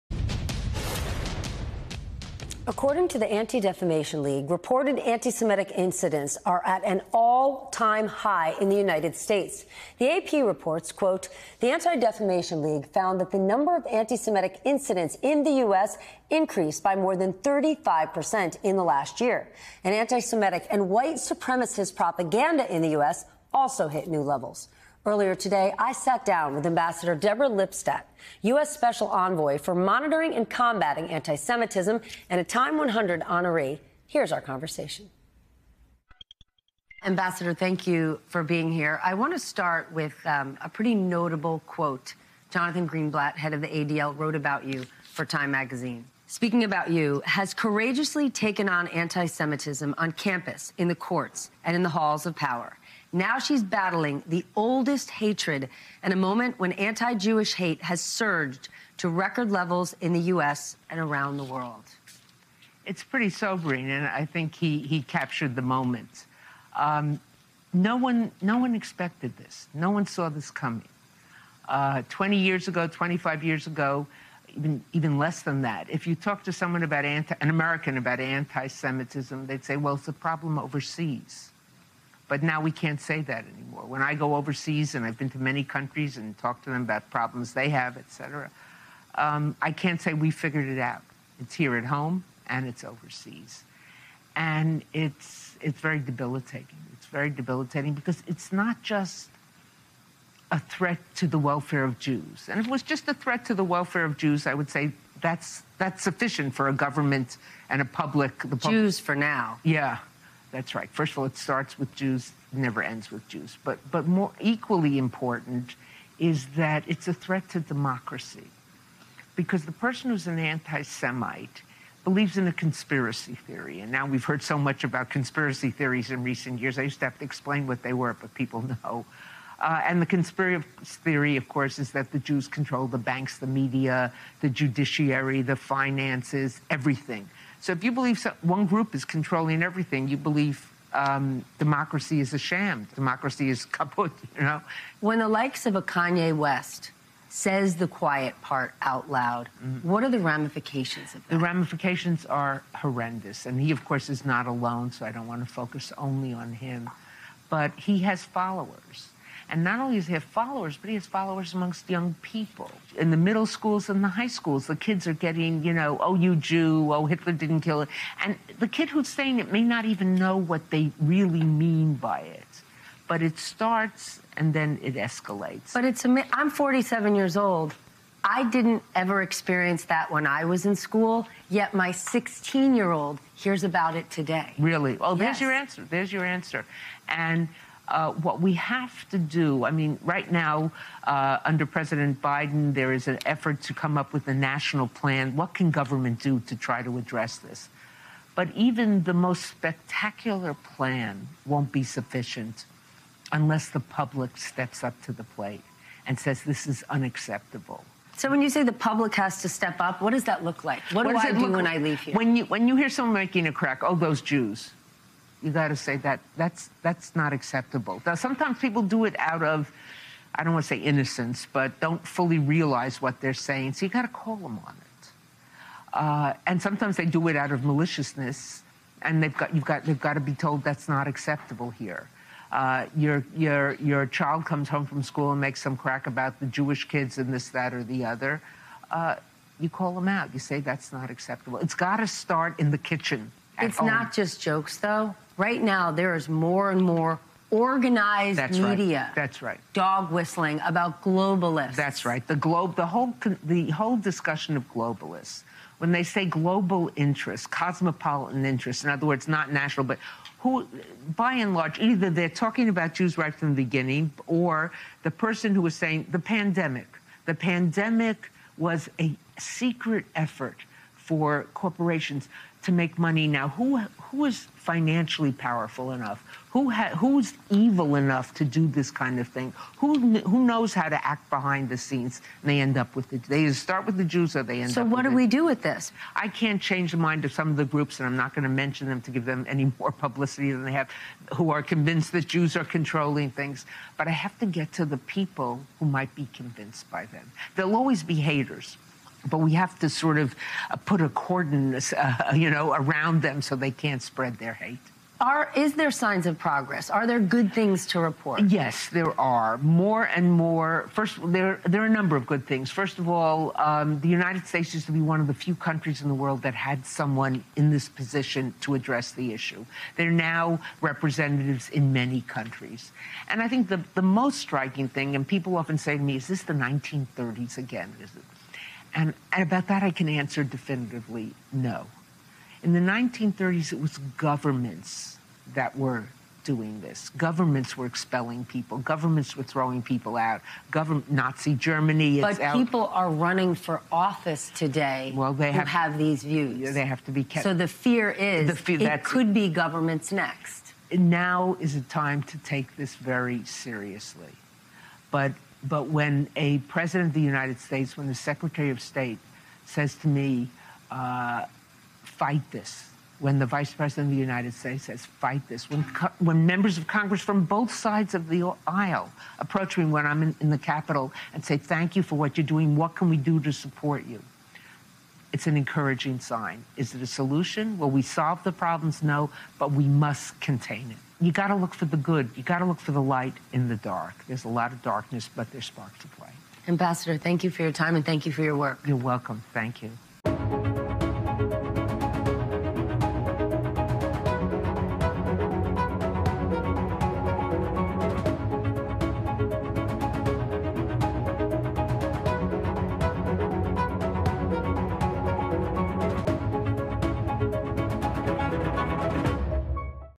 Amb. Deborah Lipstadt on monitoring and combatting antisemitism | One-on-one with Stephanie Ruhle
Stephanie Ruhle sits down with the U.S. Special Envoy for monitoring and combatting antisemitism to discuss the troubling rise of antisemitic incidents over the past year.